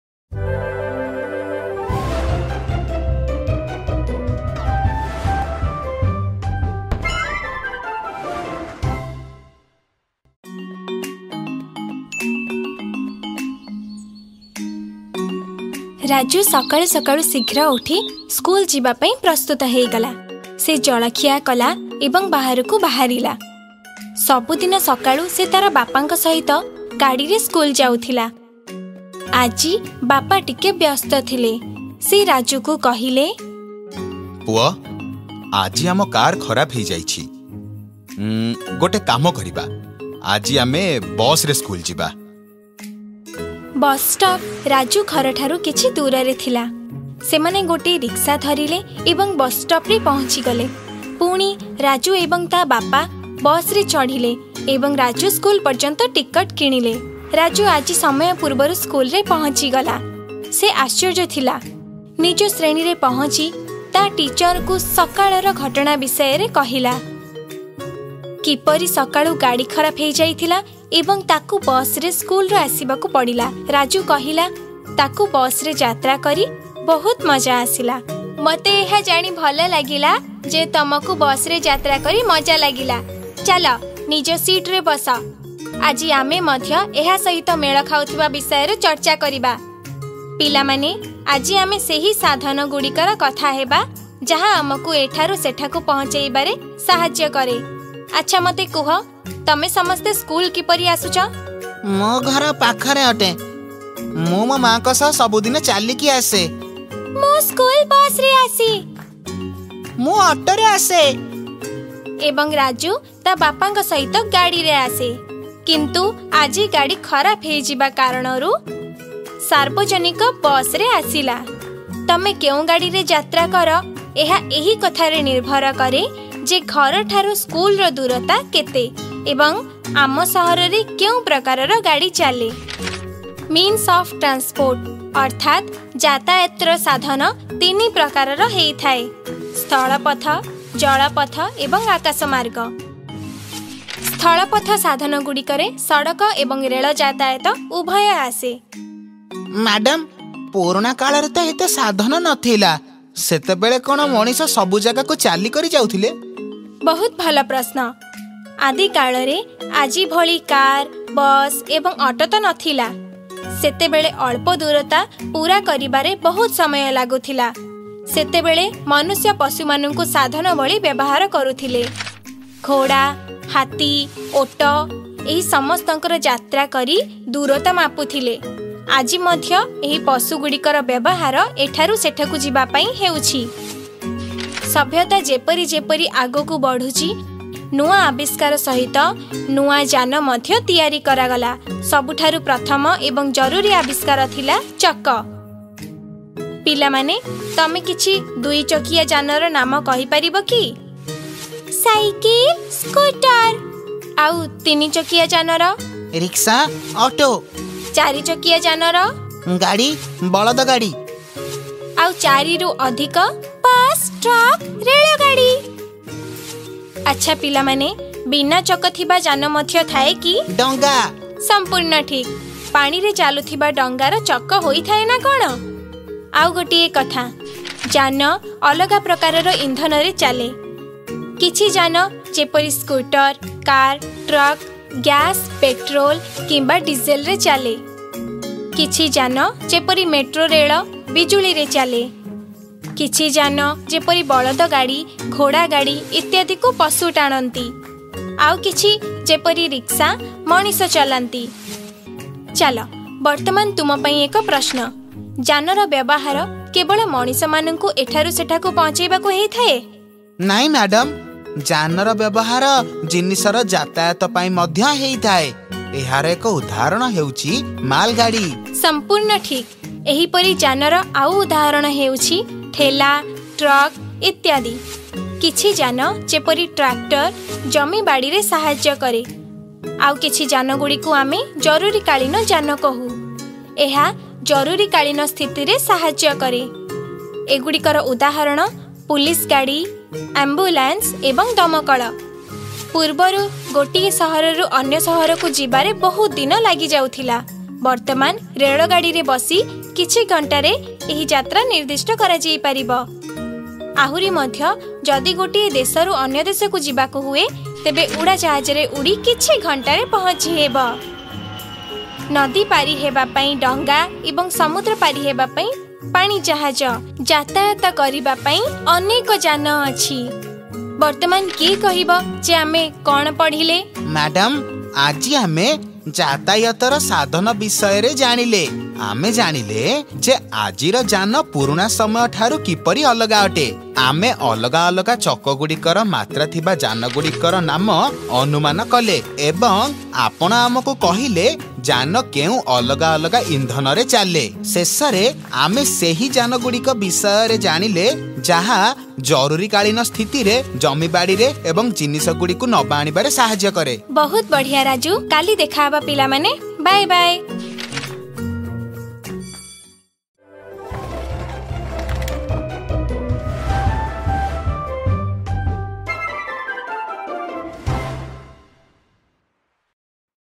ପରିବହନର ମାଧ୍ୟମ | Part 1/1 | Modes of Transport | Odia | Class 3
कक्षा 3 | NCERT | ପରିବହନର ମାଧ୍ୟମ | Part 1/1 | Modes of Transport | Odia | Class 3 | ଆମ ଚାରିପାଖରେ ଜିନିଷ

Class 3 | NCERT | From Here To There |  | Things Around us

In this video, we will study about From Here To There.